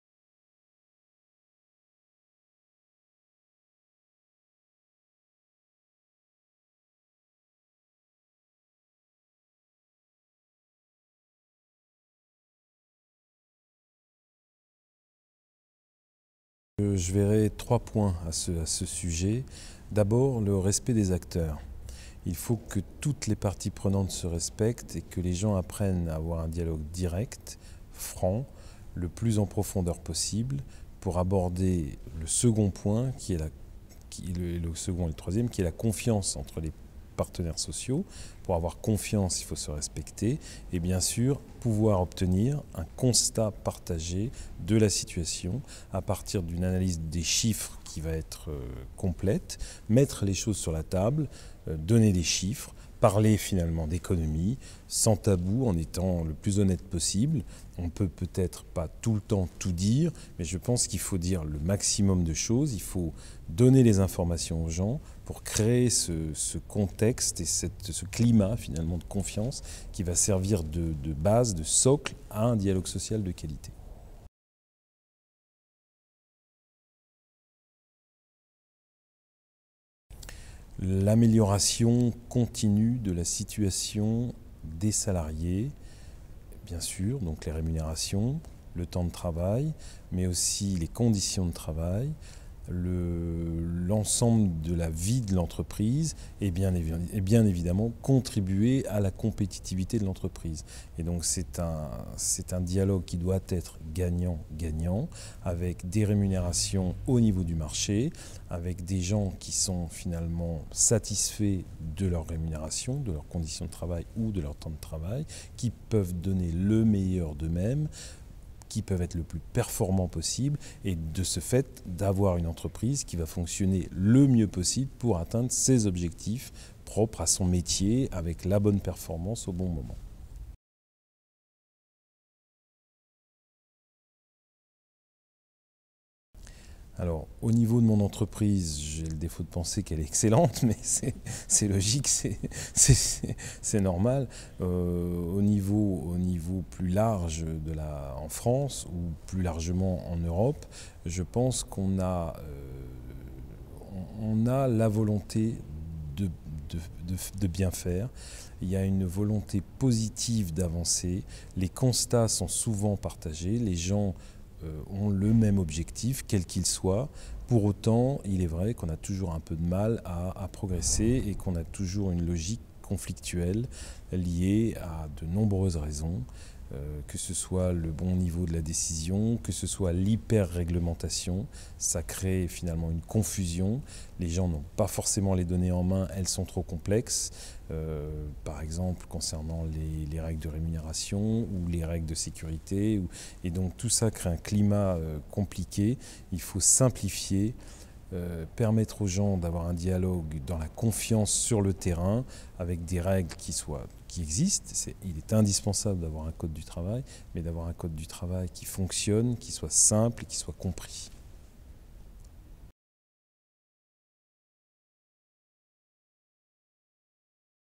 Questions à Alain EVERBECQ (Poclain Hydraulics) - cese
Questions à M. Alain EVERBECQ, Directeur des ressources humaines, Poclain Hydraulics, auditionné par la Section du travail et de l'emploi du CESE dans le cadre de la saisine gouvernementale : "Le développement de la culture du dialogue social en France".